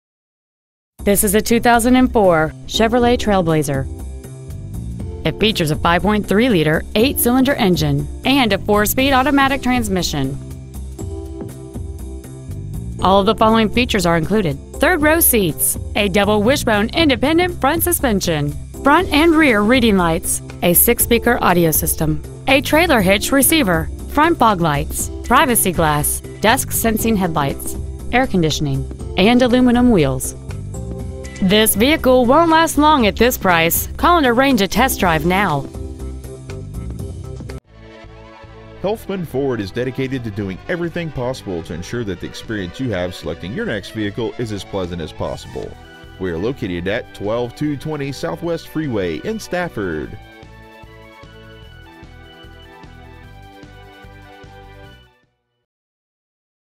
2004 Chevrolet Trailblazer Stafford TX
Year : 2004
 Make : Chevrolet
 Model : Trailblazer
 Engine : 5.3L V8 16V MPFI OHV
 Trans . : 4-SPEED AUTOMATIC
 Exterior : INDIGO BLUE
 Miles : 105,327
 Interior : PEWTER
 Stock : QE045A